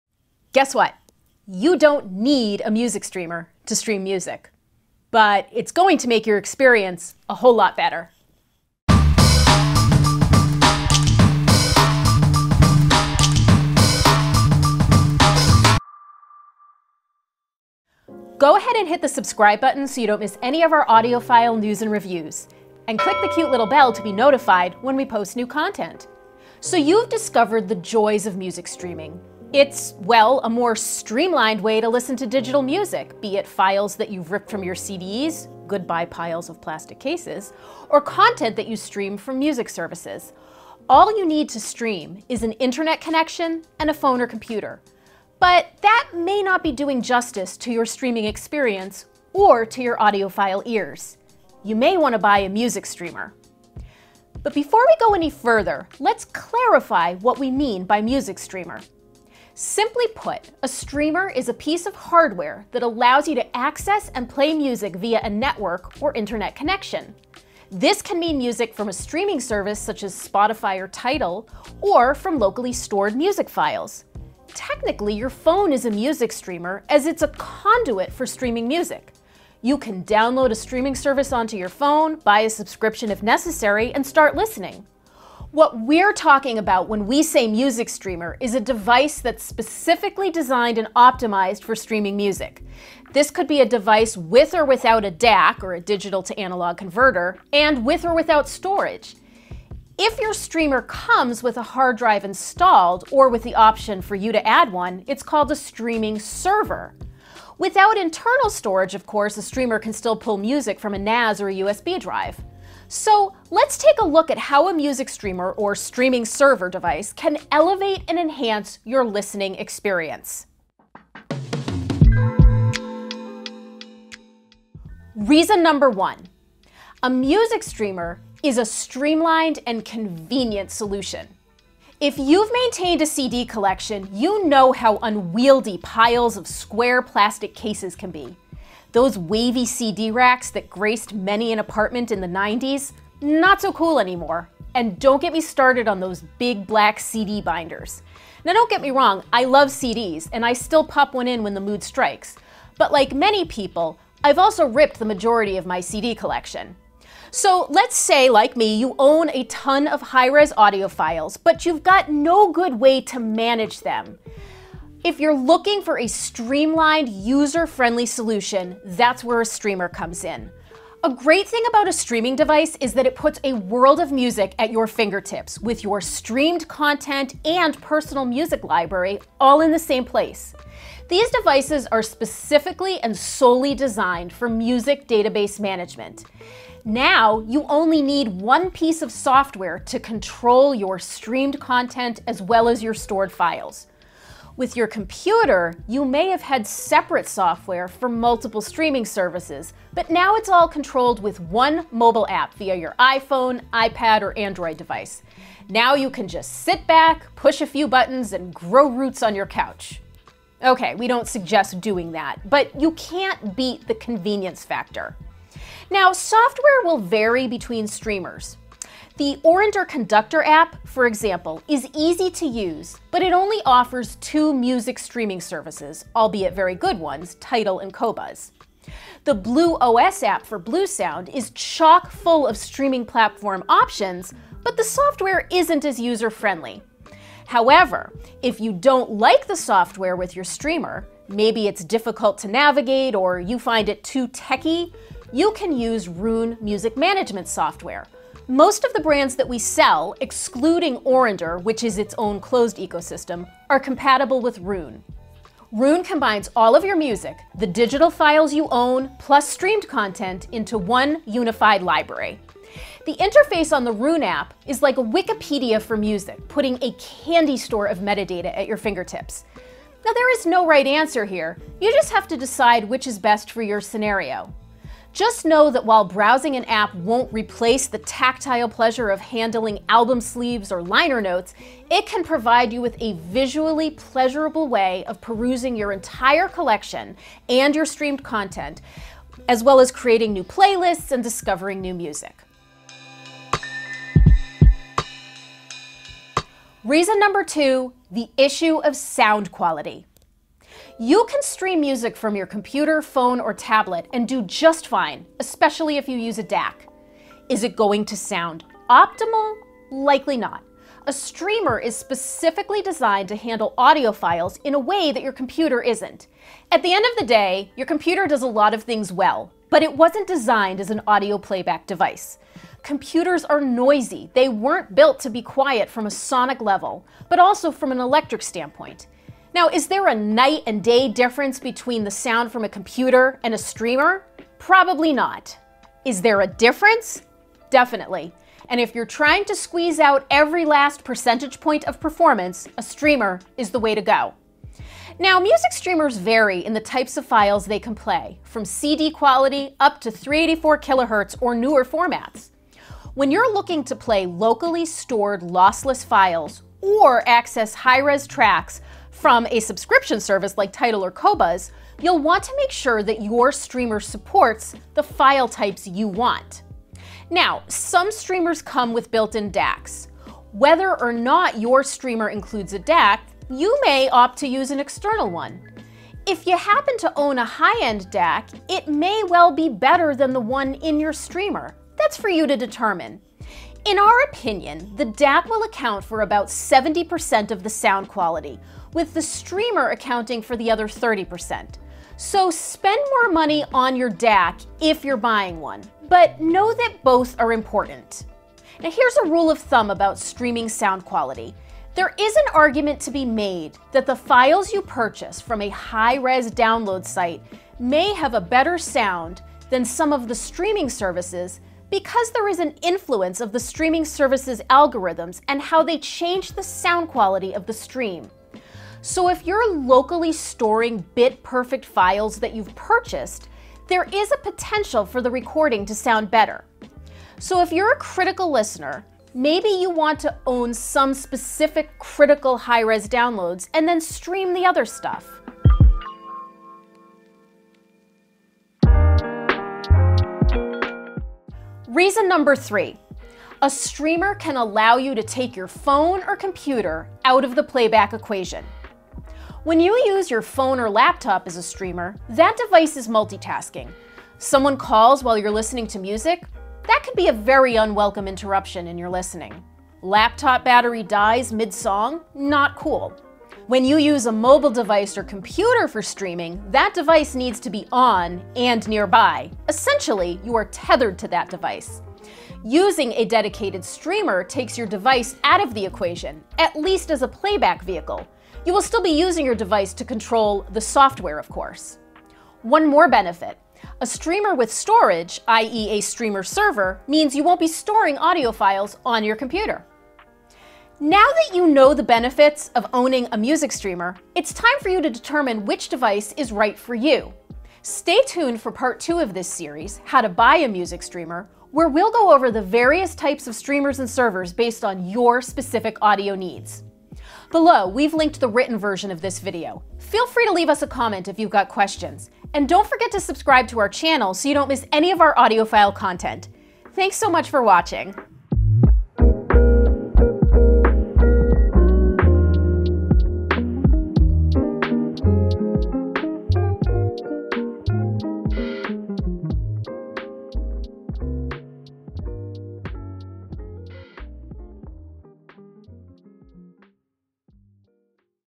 3 Reasons to Buy a Music Streamer | Moon Audio
You've decided to join the legions of Hi-Fi streaming enthusiasts, and you might be wondering whether you should purchase a music streamer. The simple fact is, you don't need a particular piece of hardware to stream music. All you need is an internet connection and a phone or computer. But that may not be doing justice to your streaming experience or to your audiophile ears...

0:00 | Introduction
0:27 | Streaming Overview
0:59 | What's a Music Streamer?
2:02 | Reason #1: Convenience
4:56 | Reason #2: Sound Quality
7:15 | Reason #3: Dedicated Hardware
8:38 | Learn More